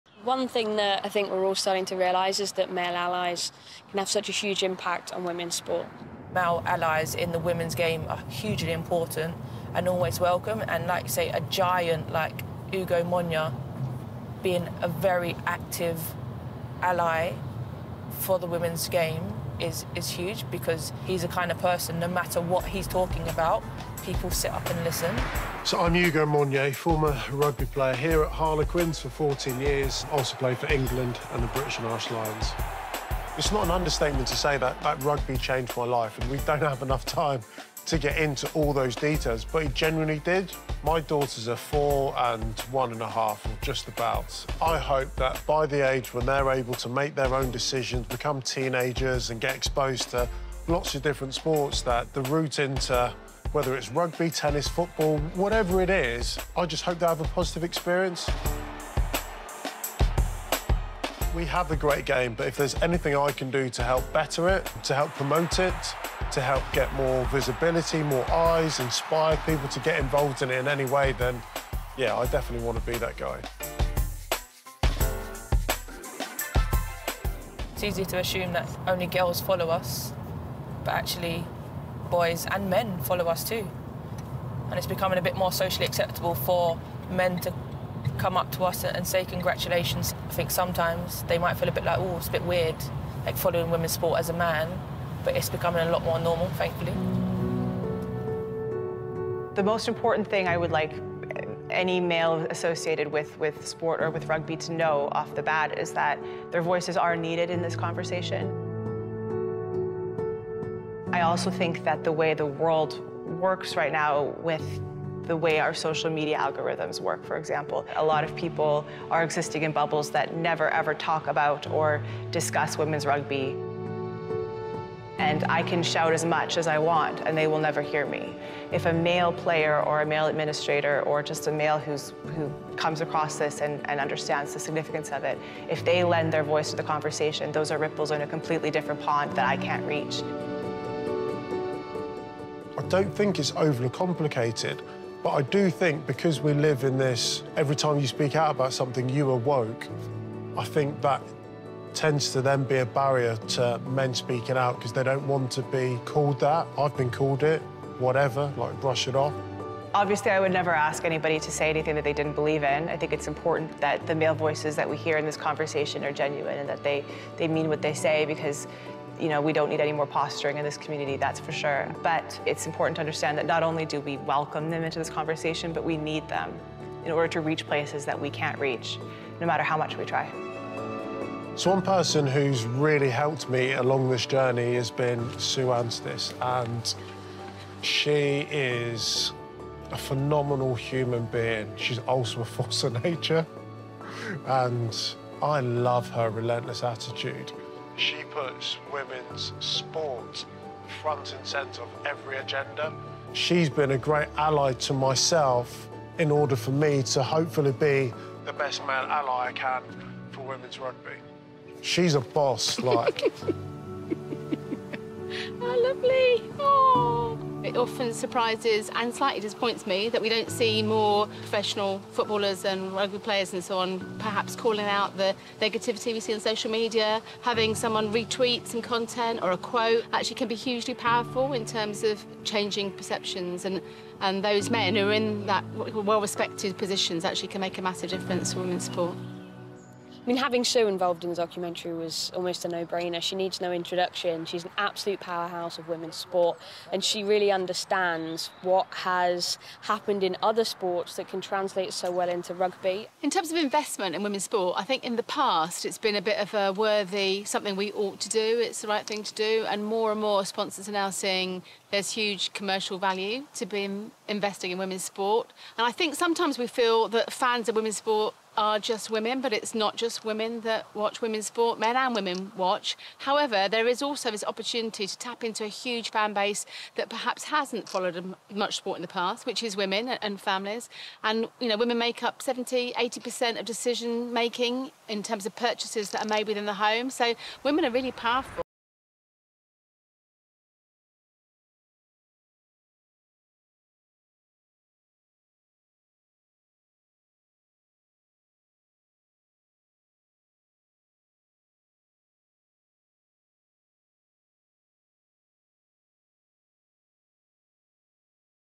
"Male Allies Have Such a Huge Impact in Women's Sport" | No Woman, No Try
The importance of women's rugby has seen a positive shift over the last few years, but there is still room for improvement. So we sit down with former England and Rugby player, Ugo Monye and the pioneer of British Women's Sport, Sue Antiss MBE, to discuss about male allies being a major factor to the rise of the women's rugby.